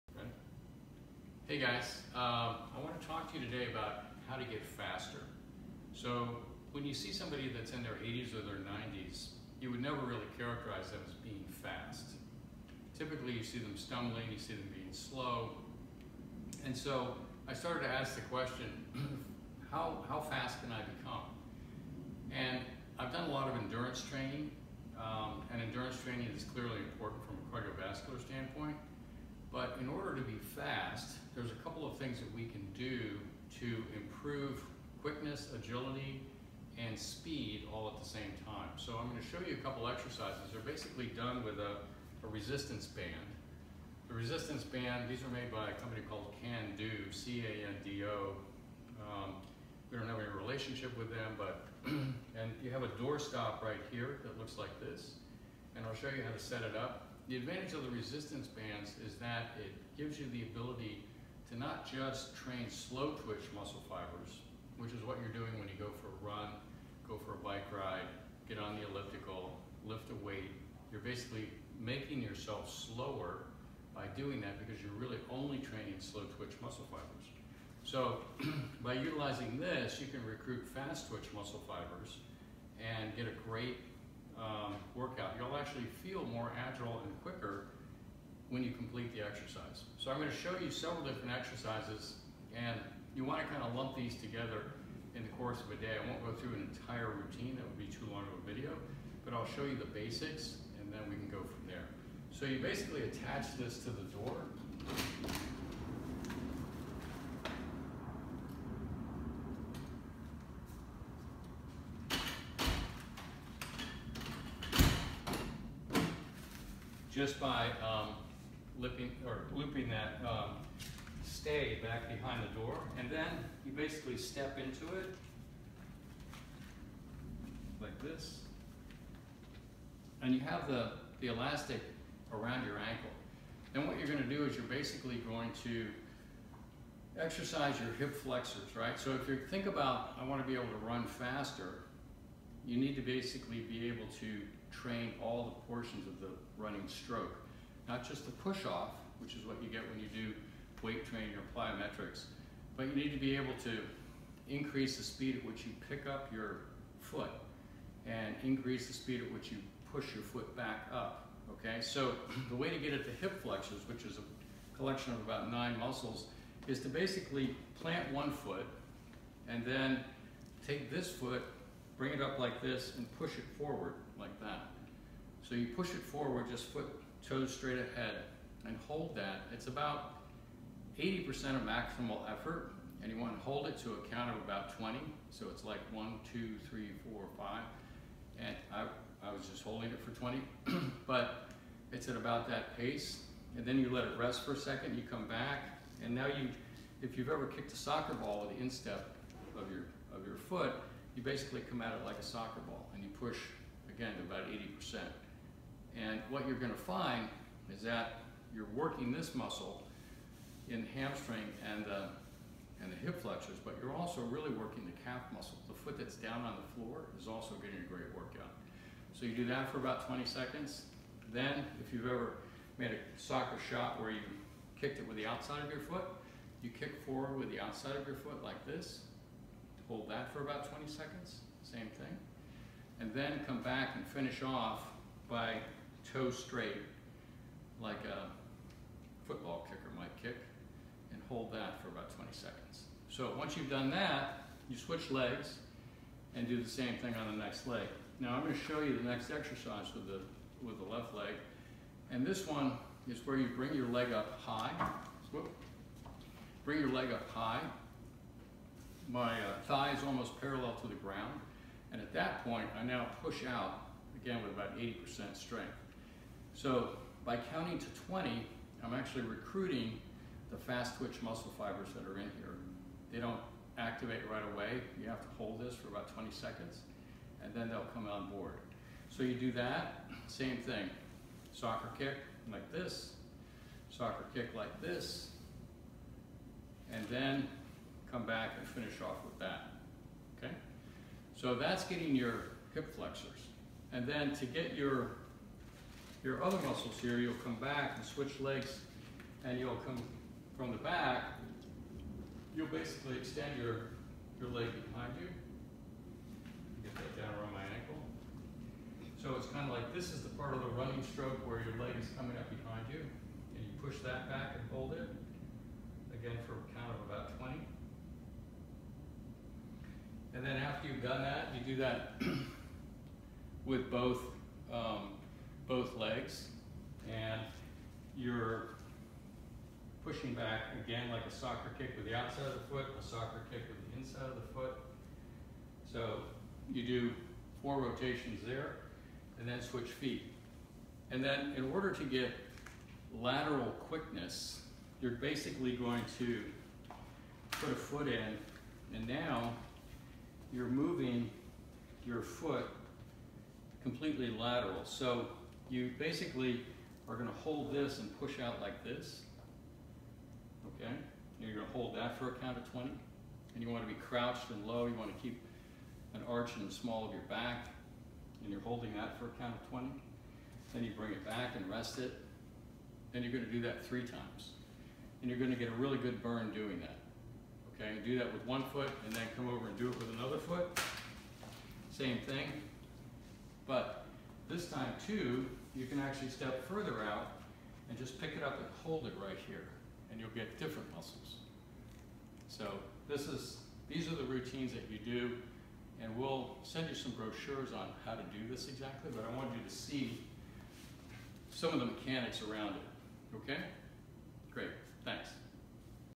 Resistance Band Instructional Video - Fast Twitch Muscle Fibers
Dr. Jeffrey Gladden demonstrates how to use resistance bands to properly engage fast twitch muscle fibers.

Gladden Longevity